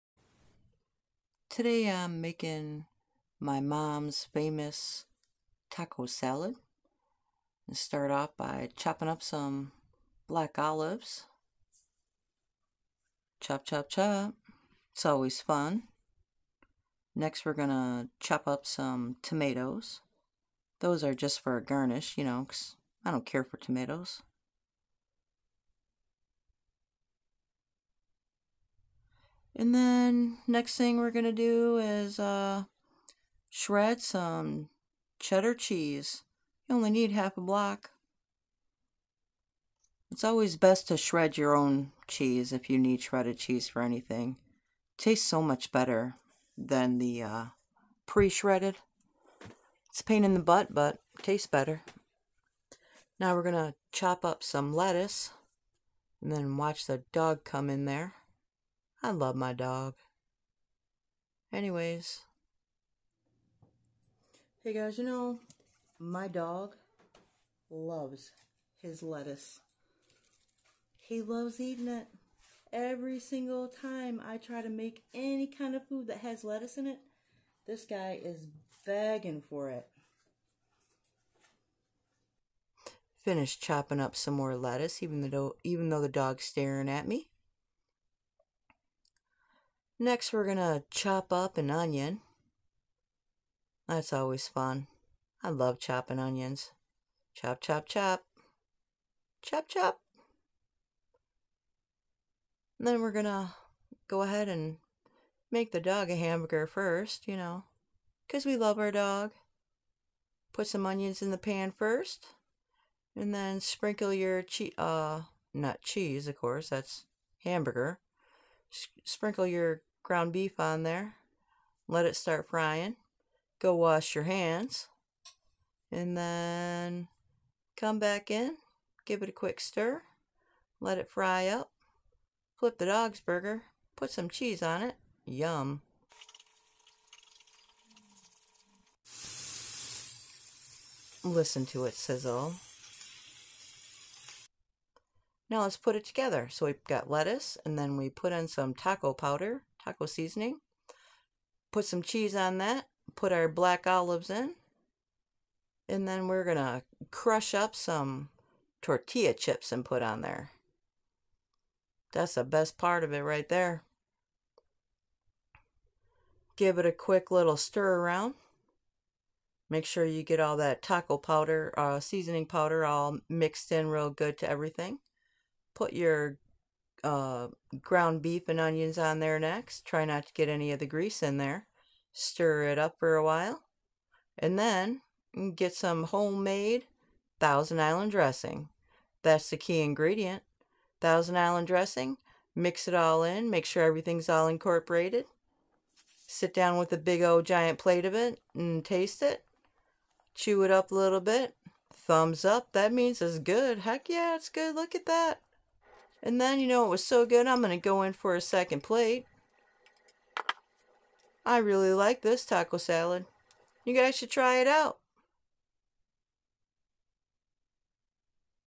Best Taco Salad Recipe Ever
This is my mom's recipe for Taco Salad. I believe she started making this recipe in the mid 1980s. Since then everyone in the family would request her to make this when they'd be coming over to hangout.

Ingredients
1 lbs ground beef
1/2 medium onion - chopped
1 can black olives - sliced
1/2 block cheddar cheese - shredded
1 medium tomato - chopped
1/4 - 1/2 head lettuce - shredded
tortilla chips - crushed
taco seasoning - about 3/4 envelope
thousand Island dressing
Directions -
fry up the burger with the onion. Put the lettuce in the bowl first. add the seasoning. add the cheese, black olives, add a few handfuls of crushed tortilla chips. mix to combine these ingredients. add the fried burger and onions. add the dressing (as much or as little as you want) mix well. Eat. 👍

If you try out my mom's Taco Salad let me know how it turned out for you in the comments below.